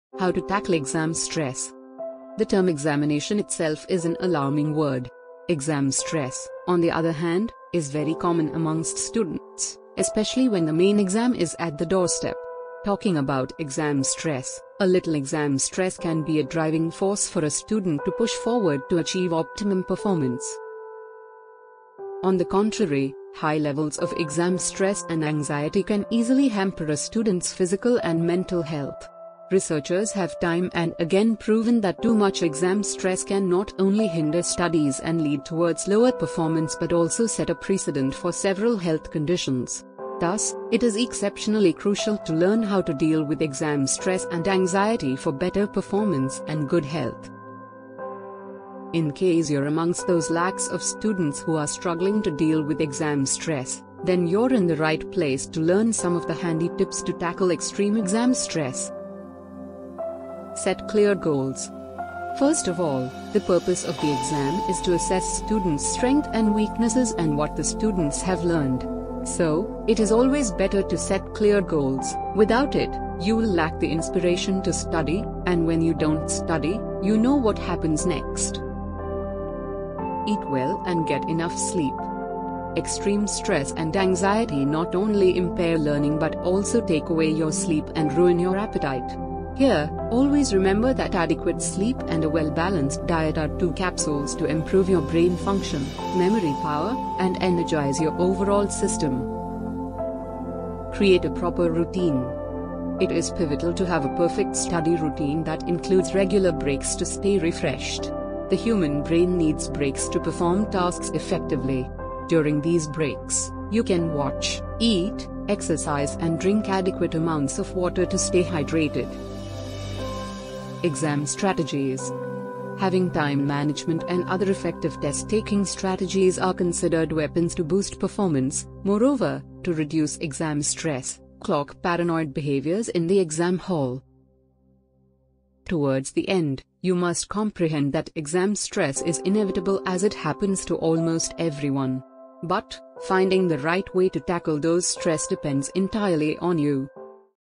How to Tackle Exam Stress? | Tips to tackle extreme exam stress | Best Trainers on UrbanPro
Extreme exam stress and anxiety can not only hinder studies and lead towards lower performance but also set a precedent for several health conditions. So, watch this video until the end and learn some of the handy tips to tackle extreme exam stress for better performance and good health.